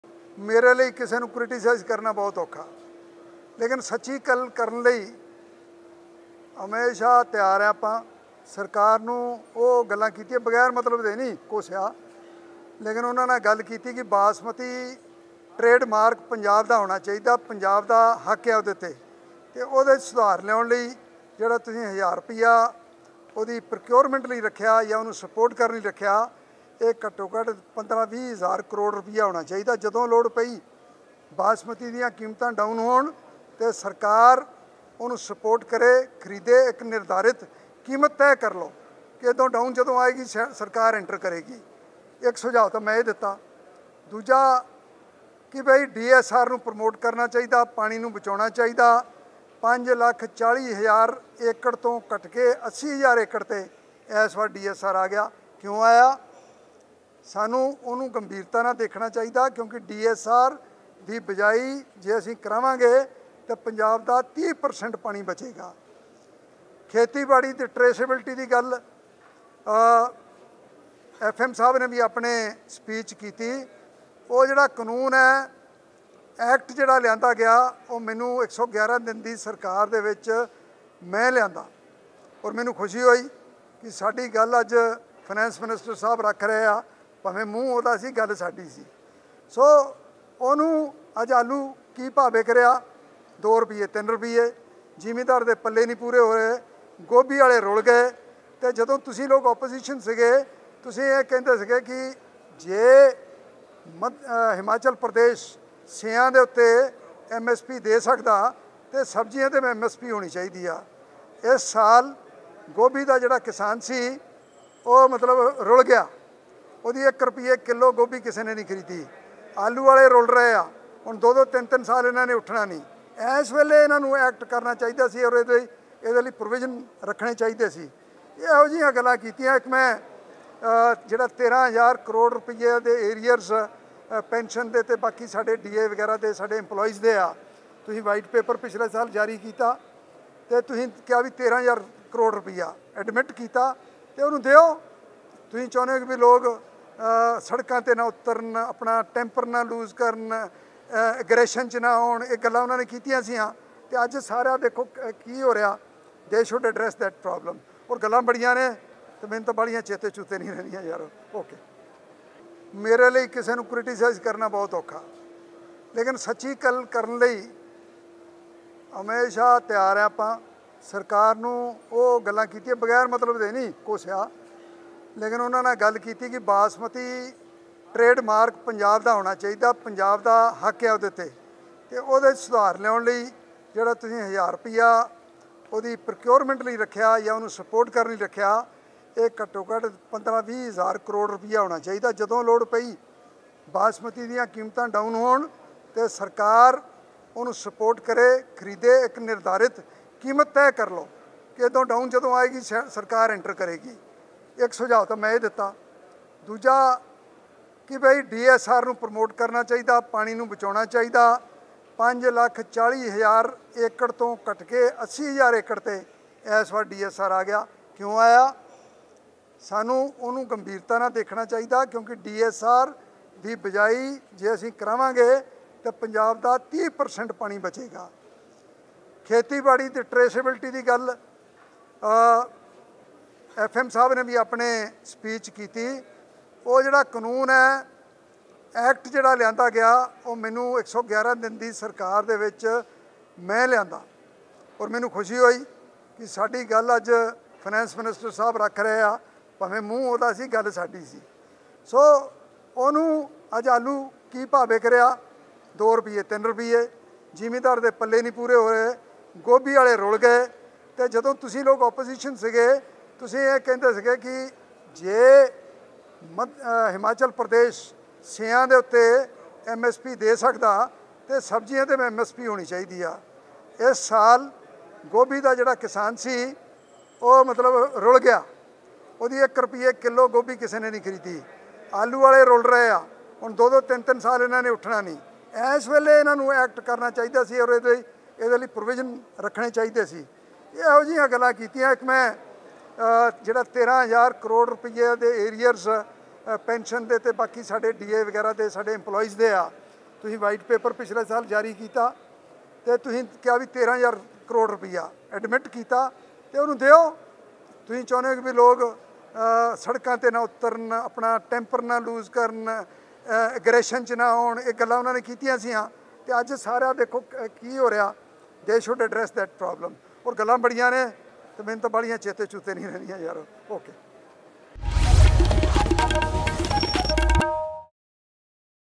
''ਮੇਰੇ ਲਈ ਕਿਸੇ ਨੂੰ criticize ਔਖਾ,ਪਰ ਸੱਚੀ ਗੱਲ ਕਹਿਣ ਲਈ ਮੈਂ ਹਮੇਸ਼ਾ ਤਿਆਰ'',Rana Gurjeet Singh
VidhanSabha